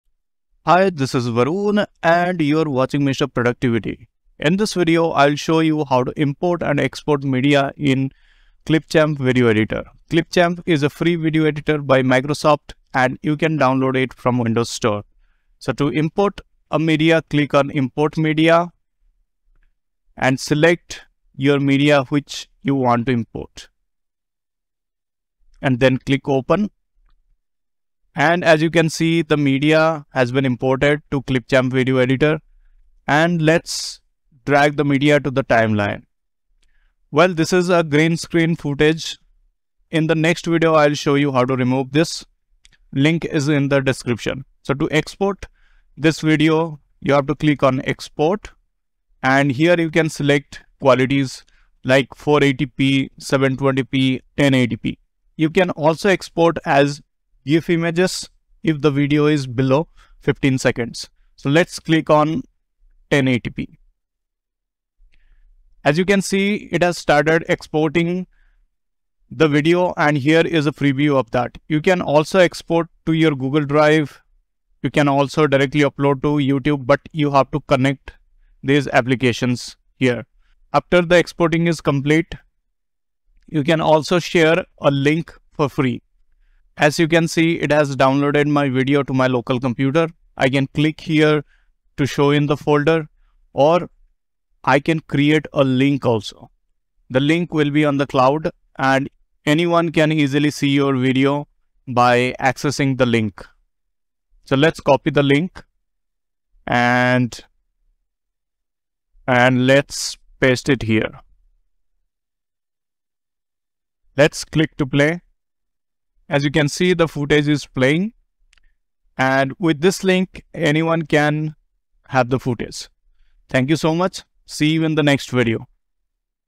Master File Import and Export in ClipChamp: The Secret to Effortless Video Editing!
ClipChamp video editing tutorial: Learn how to import and export files like a pro! In this tutorial, we will show you how to efficiently import and export files in ClipChamp video editor. From importing footage to exporting your final edit, we will cover all the essential steps and tips you need to know to make your video editing process a breeze. Follow along as we guide you through the process step by step and show you how to make the most of ClipChamp's file import and export features.

We would like to thank the original creator for allowing us to use their footage in our video.

Picture credits Text-to-Speech AI Videos